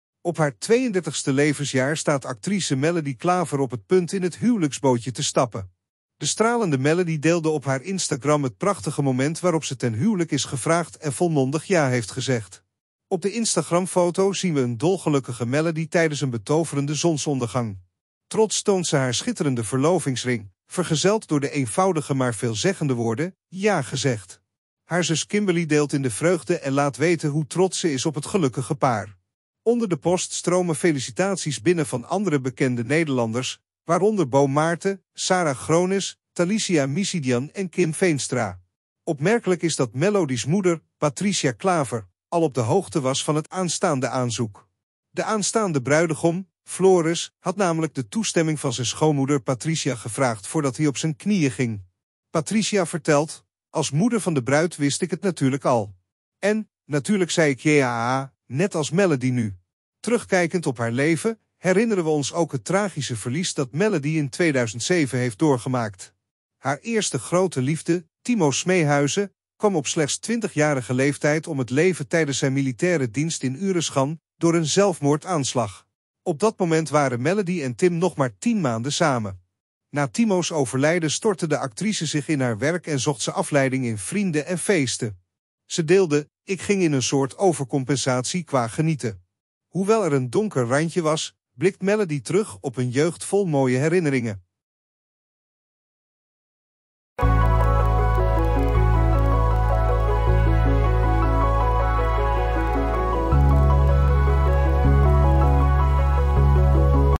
Melody Klaver verrast met romantisch huwelijksaanzoek!
Bekijk het emotionele moment waarop de getalenteerde Nederlandse actrice Melody Klaver (32) 'ja' zegt tijdens een prachtig huwelijksaanzoek, gedeeld op haar Instagram-account. De foto's tonen een stralende Melody bij zonsondergang met haar schitterende verlovingsring. Haar zus Kimberley en andere bekende Nederlanders feliciteren het gelukkige paar. Ontdek ook het bijzondere verhaal van Melody's moeder, Patricia Klaver, die al op de hoogte was van het aanzoek. Blijf op de hoogte van het laatste nieuws over Melody Klaver en haar aankomende huwelijk.